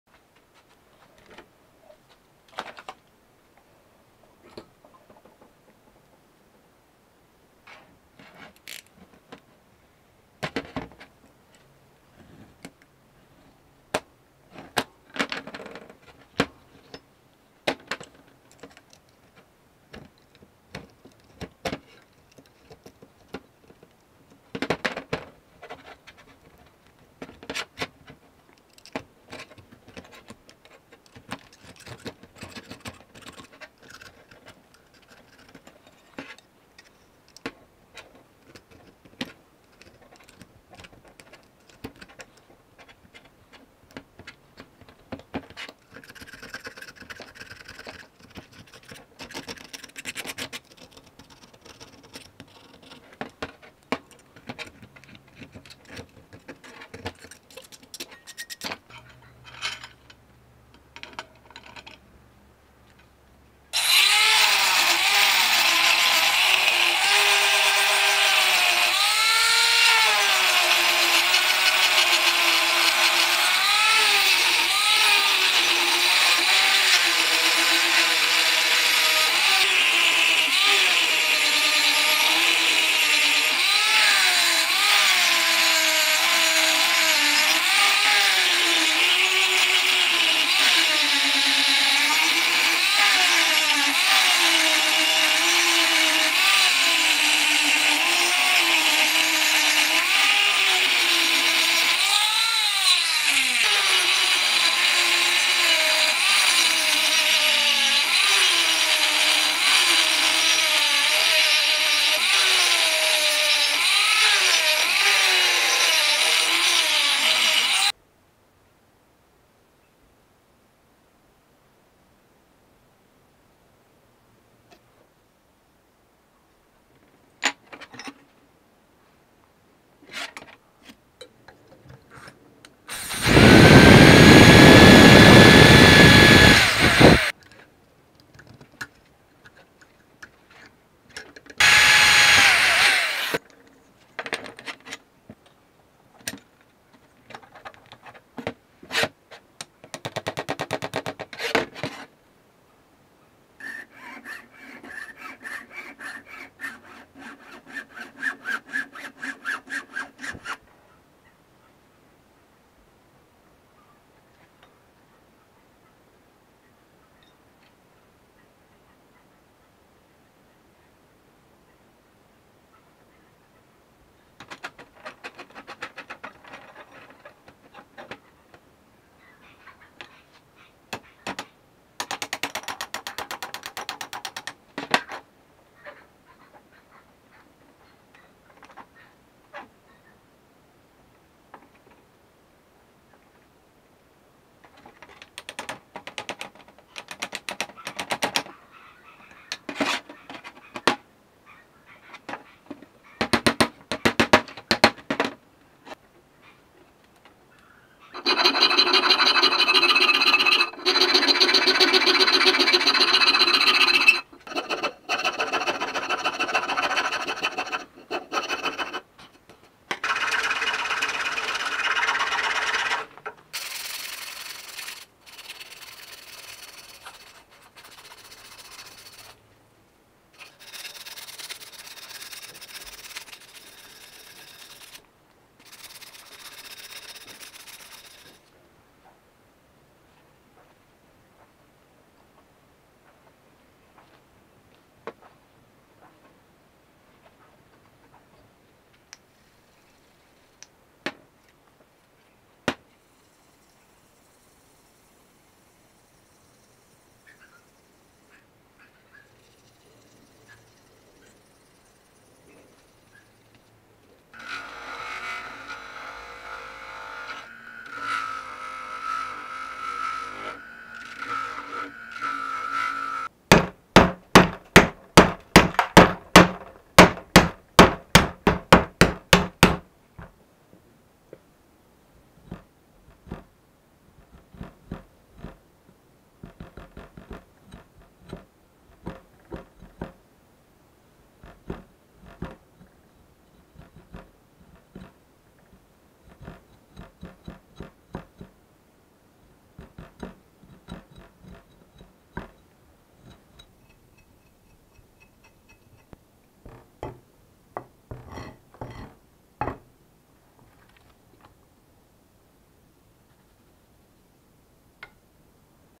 Meat cleaver restoration
Here is my first attempt on restoring a meat cleaver, I know I should polish it up a little more, but I ran out of time, and my shop is freezing cold right now, so enjoy! :)